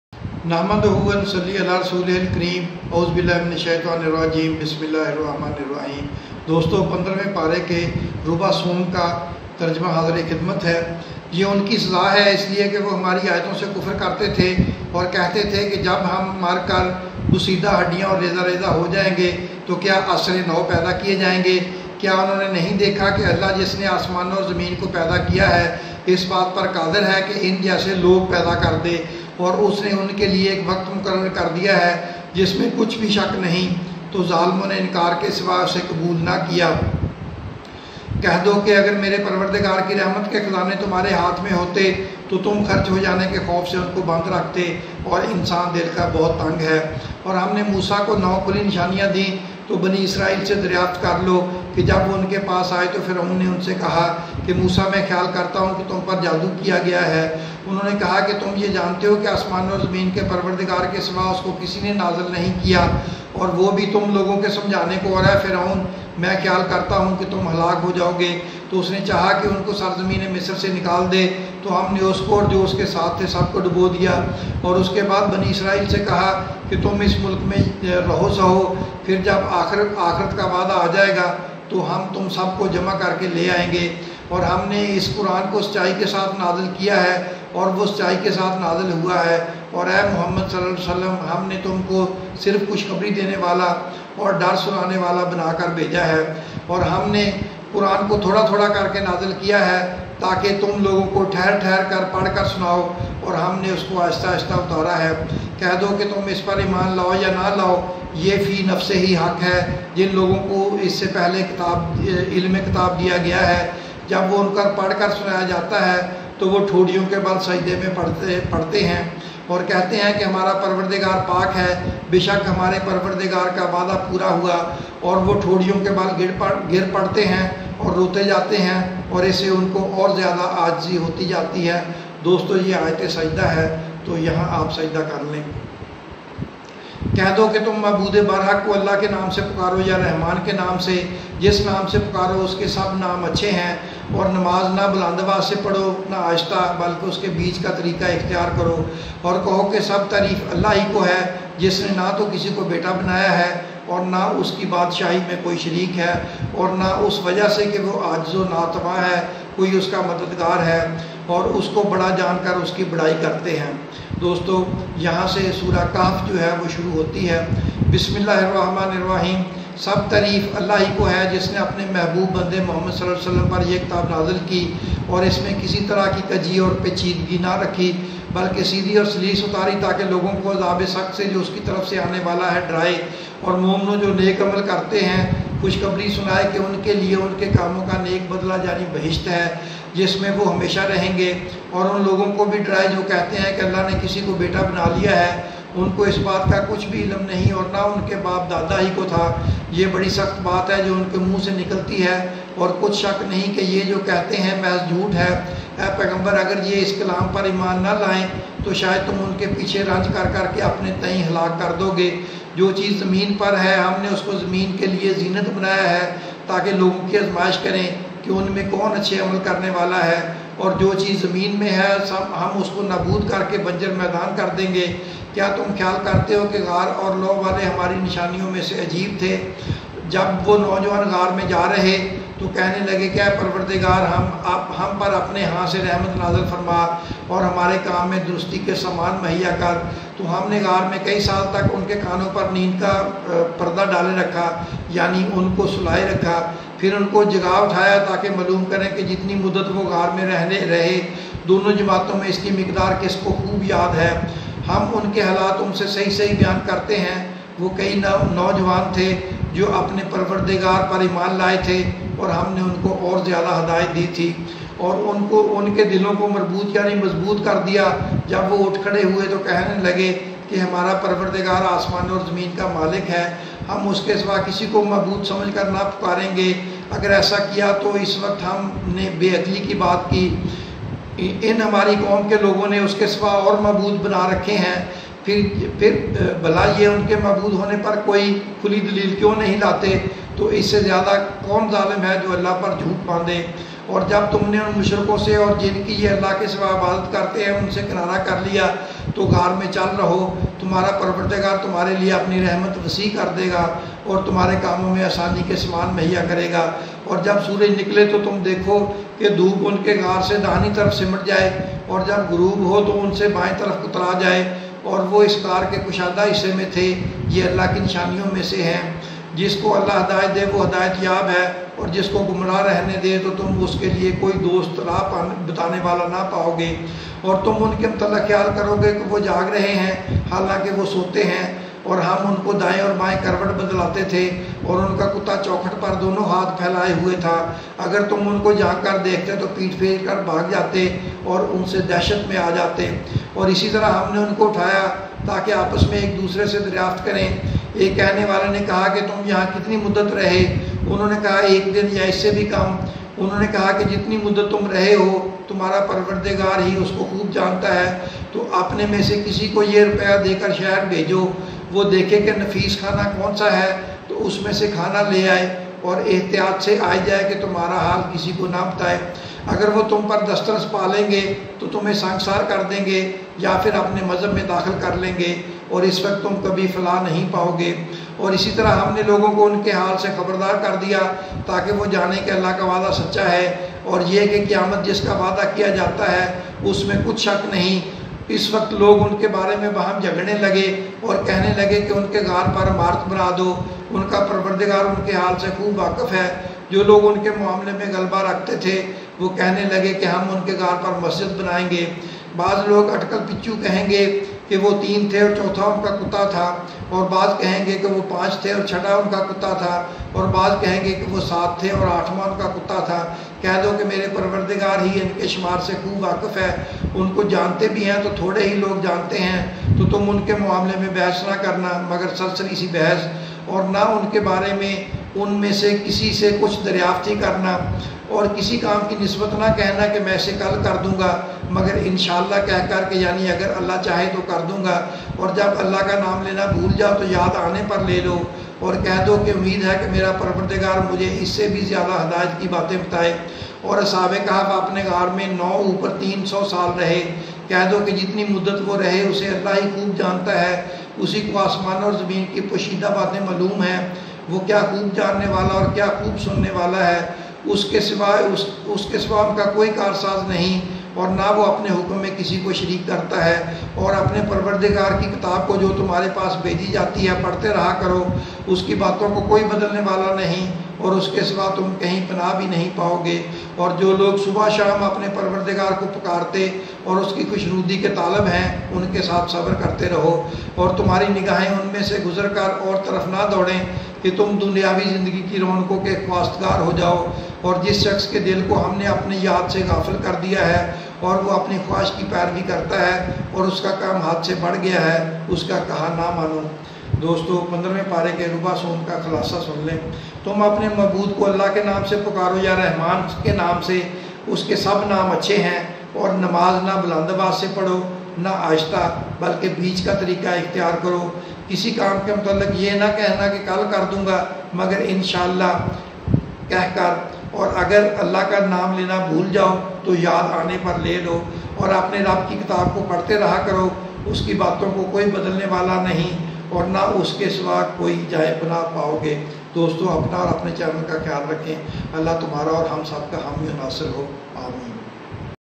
Para 15, quarter 3 (Para 15, Ruba 3)
In this episode, Urdu translation of Para fifteen, quarter three is presented.